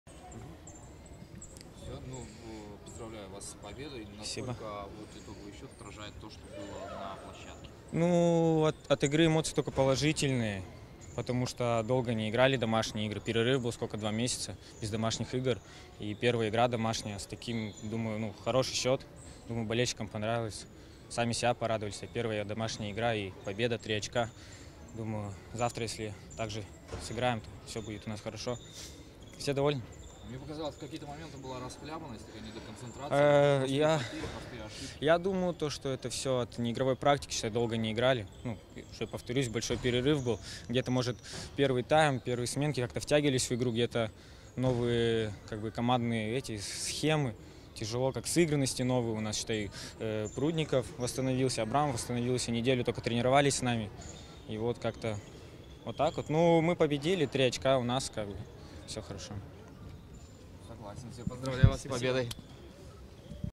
Егор Иванов, игрок МФК "Синара"
Париматч-Суперлига. 8 тур. Синара - Новая генерация. 8-3. Матч №1